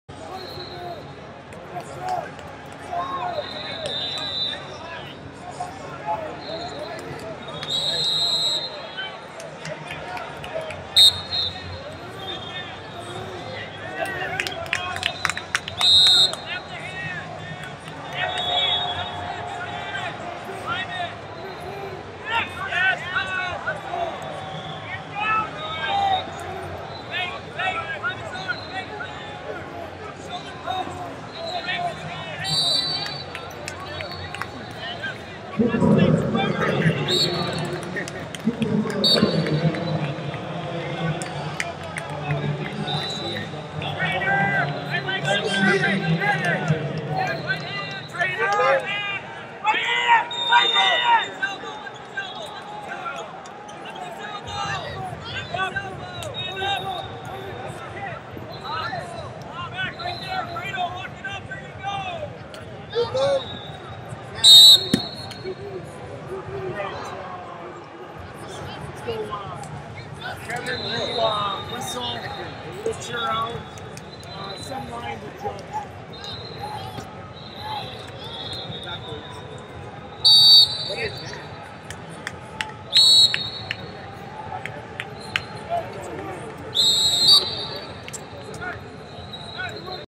170 Lbs Consi Of 32 #2 - David Malin, Wisconsin Vs Nicholas Fox, Iowa B563
2022 USMC/USAW Junior And 16U National Championships - 170 Lbs Consi Of 32 #2 - David Malin, Wisconsin Vs Nicholas Fox, Iowa B563